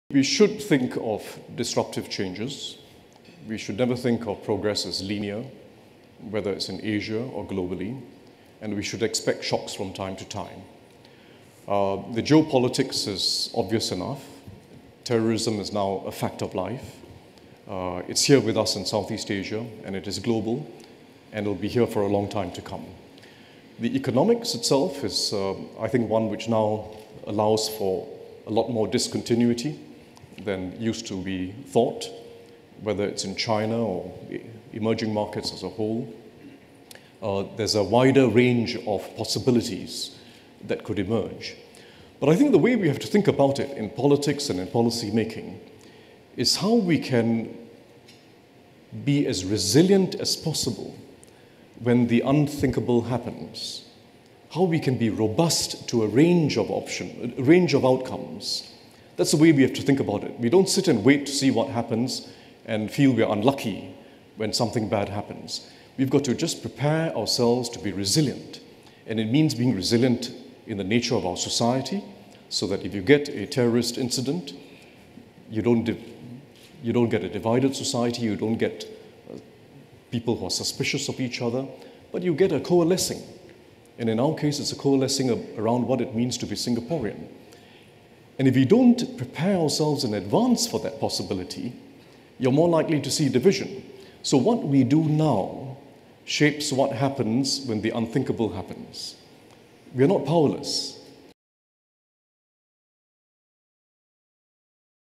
4. On preparing for the unthinkable
Deputy Prime Minister Tharman Shanmugaratnam commenting on preparing for the unthinkable. He was speaking at a dialogue session at the welcome dinner of The Singapore Summit 2016 on 16 September 2016. The session was moderated by international broadcaster Nik Gowing.